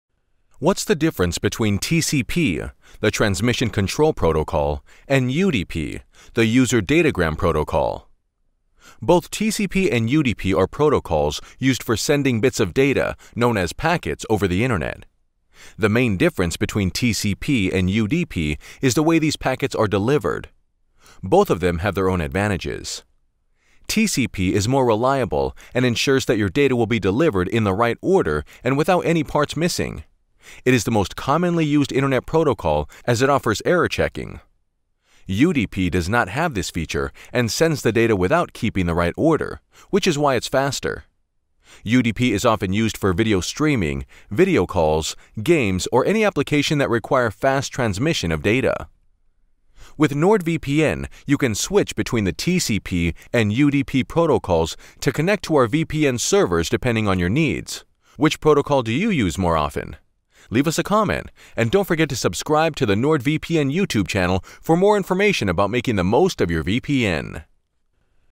TCP vs UDP. Which to use? | NordVPN
📱 Connect to OpenVPN with TCP or UDP 🔒
🚀 The world’s leading VPN with a Threat Protection feature comes with a huge discount.

The speed and the quality of your data transfers depend on which network protocols you use, UDP or TCP. What are TCP and UDP? How does OpenVPN perform with UDP vs TCP protocols? Refresh your knowledge and find out the difference between TCP and UDP.

► RECOMMENDED VIDEOS ►

Contents:

00:00 Intro
00:09 What are UDP and TCP?
00:15 The difference between UDP vs. TCP
00:50 UDP and TCP on NordVPN

What are TCP and UDP?
TCP (Transmission Control Protocol) is a network protocol that moves your data over the internet from your device to a web server. You use the TCP protocol whenever you chat to your friends online, send emails, or browse the web.

UDP stands for User Datagram Protocol. Compared to TCP, the UDP protocol is less trustworthy but faster and more straightforward. It’s valuable in situations where higher speeds are crucial, like in streaming or gaming.

How TCP and UDP work

TCP is more reliable than UDP. It transfers your data packets from your device to a web server. UDP is faster and simpler, but it doesn’t guarantee the delivery of your data.

What is the main difference between TCP and UDP?

UDP is faster than TCP, because the user doesn’t have to allow or acknowledge receipt to resent the data. This lets UDP establish connections and transfer data faster. However, this also raises some concerns whether UDP is actually safe. In terms of the UDP vs TCP VPN debate, OpenVPN works best on a UDP port, although it can be configured to run on any port.

Check out NordVPN’s German and French channels  ⏯️ 🌐